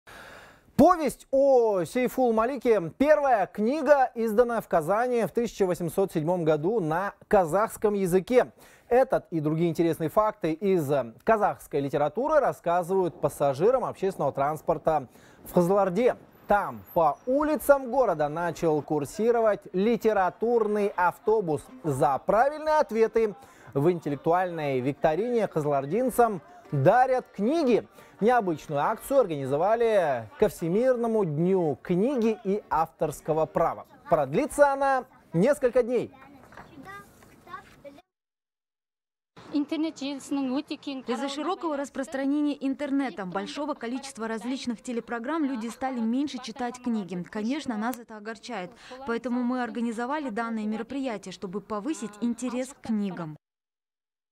«Литературный автобус» запустили в Кызылорде
Повесть о «Сейфул-Малике» – первая книга, изданная в Казани в 1807-м году на казахском языке. Этот и другие интересные факты из казахской литературы рассказывают пассажирам общественного транспорта в Кызылорде. Там по улицам города начал курсировать литературный автобус, рассказывает «Хабар 24».